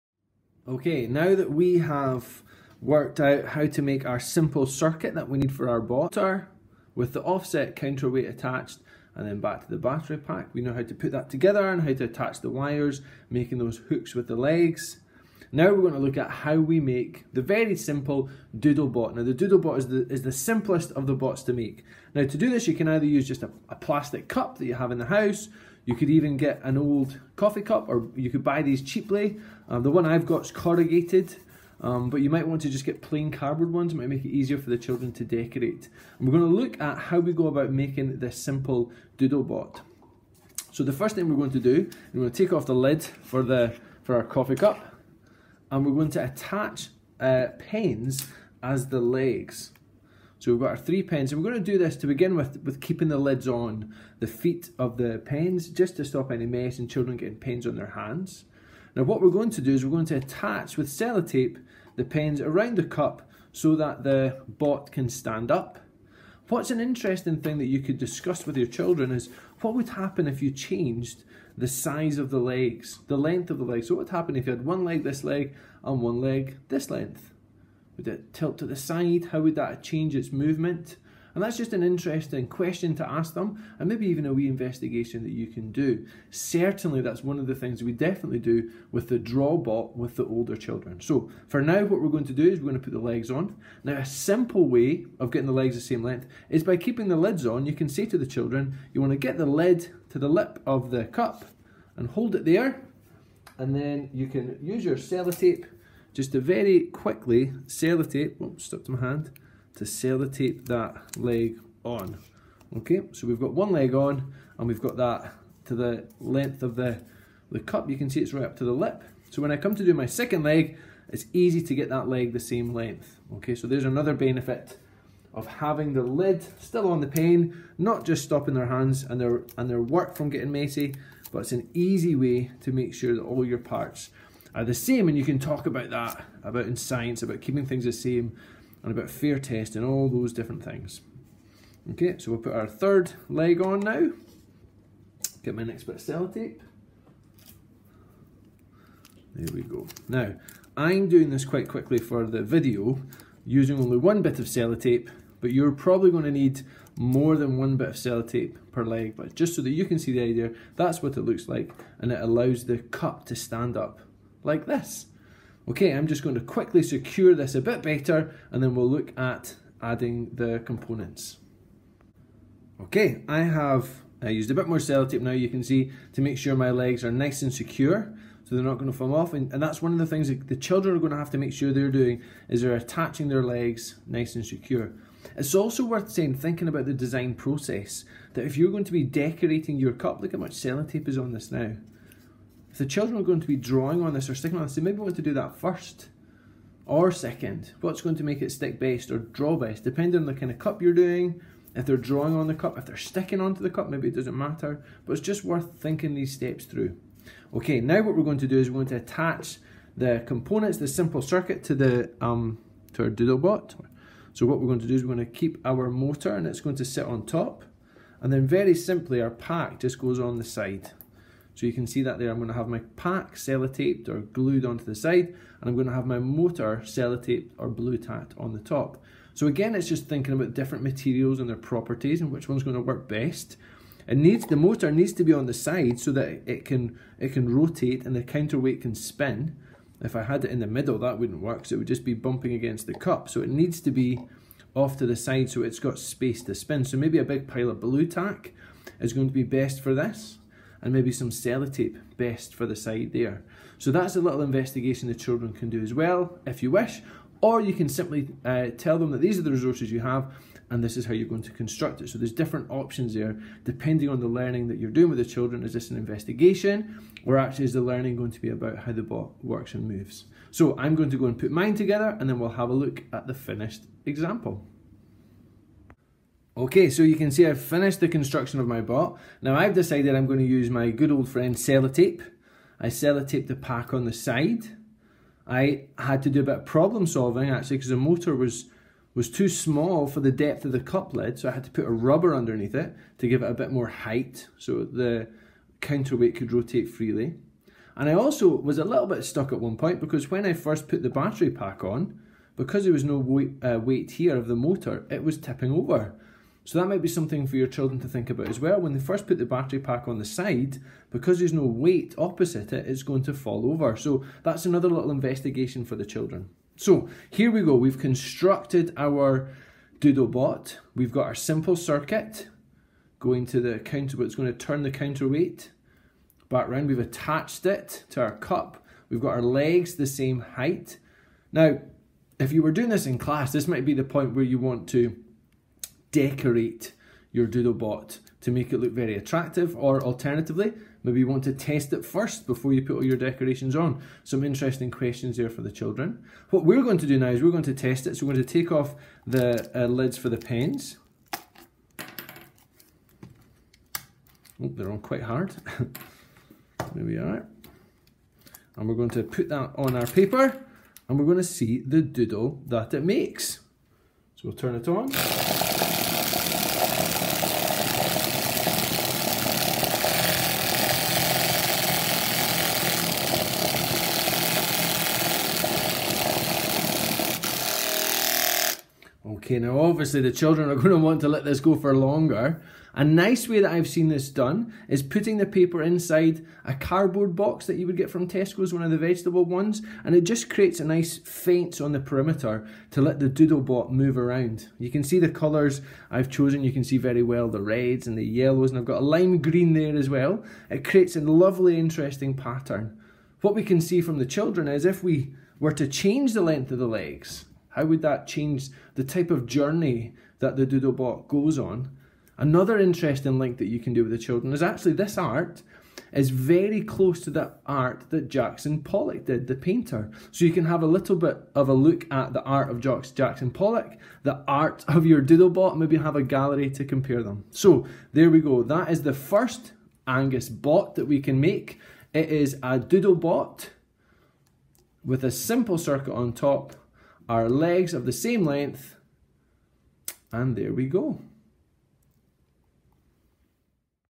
How to make a 'Doodle Bot'
This video will demonstrate how to make a Doodle Bot.

You can easily order the parts required by entering the following product codes
5 bots = 70-6047
30 bots = 70-6049